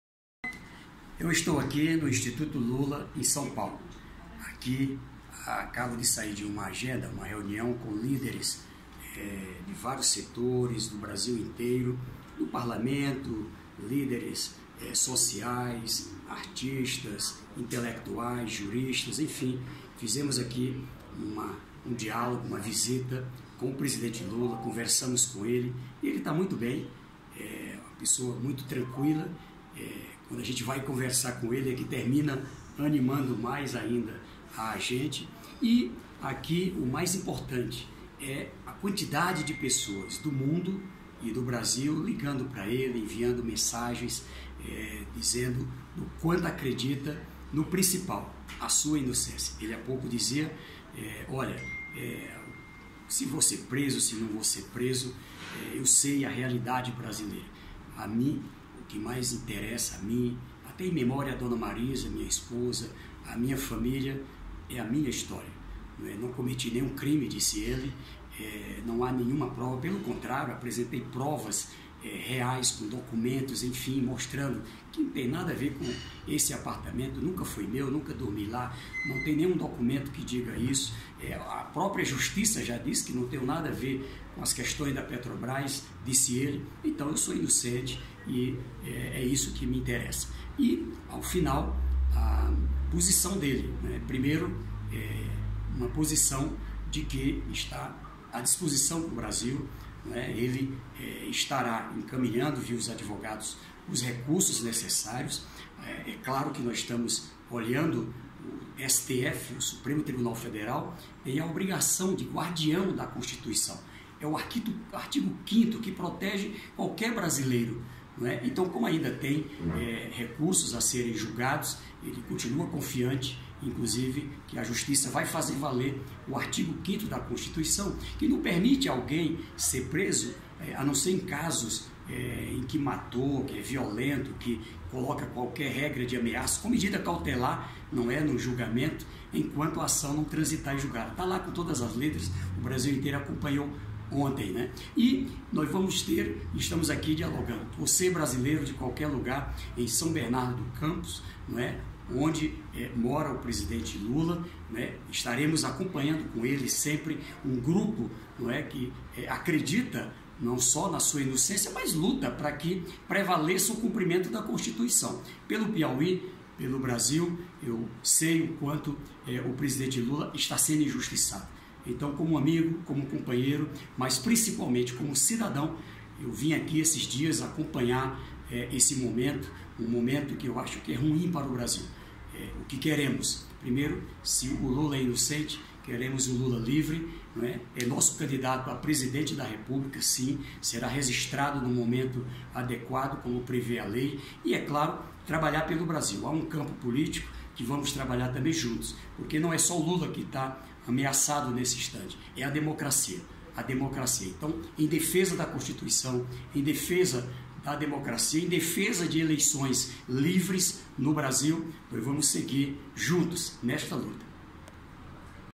Wellington Dias publica vídeo em apoio a Lula
O governador do Piauí Wellington Dias (PT), que acompanhou o julgamento do habeas corpus de Luiz Inácio Lula da Silva, publicou em suas redes sociais um vídeo em apoio do ex-presidente. Wellington Dias esteve nesta quinta-feira, (05/04), em reunião no Instituto Lula em São Paulo.